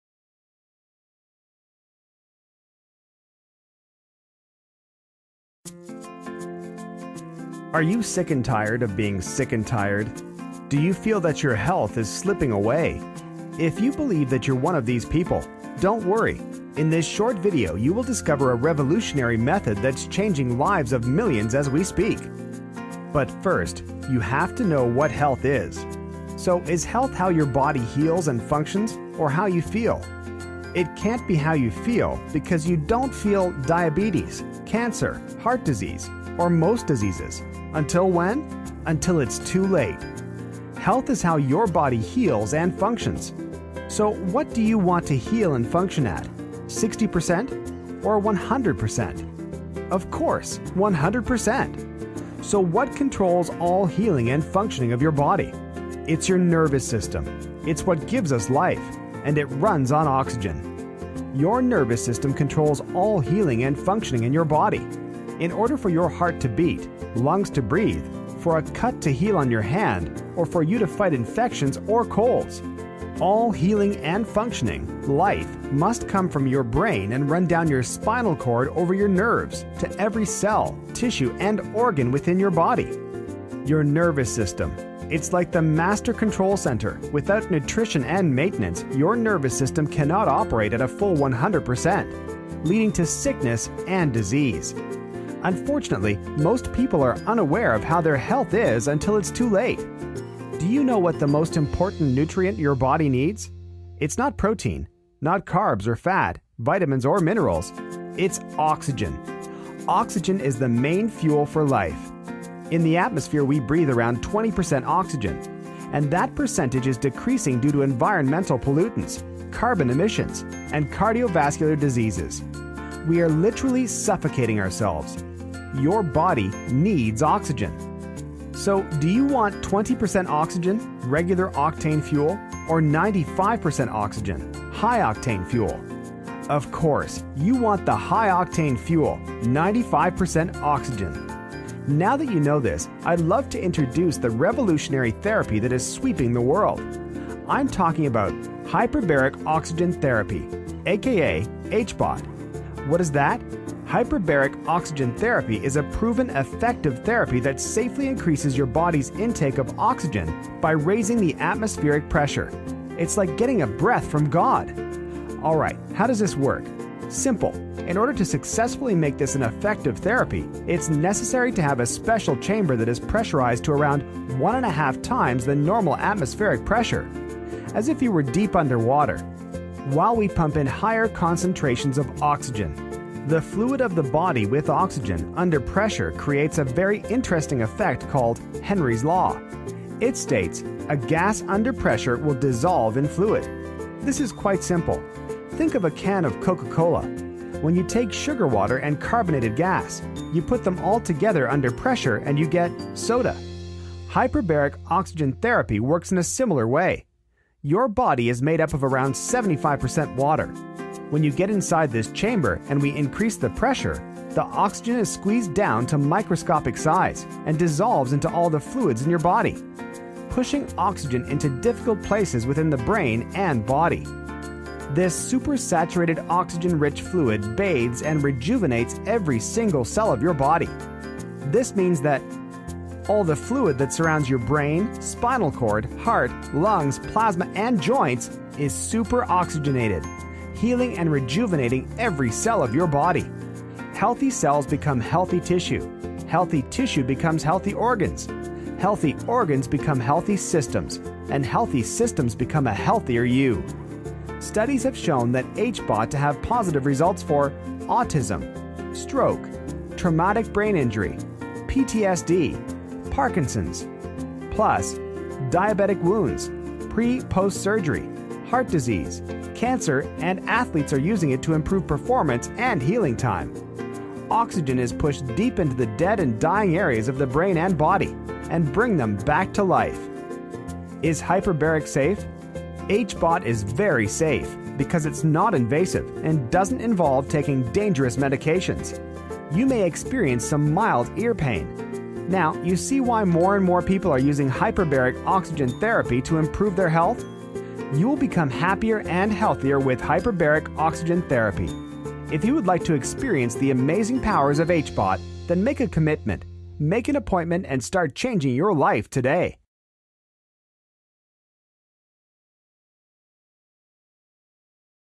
Waarom Milde Hyperbare Zuurstof Therapie?
Engelstalige animatie over milde hyperbare zuurstof therapie.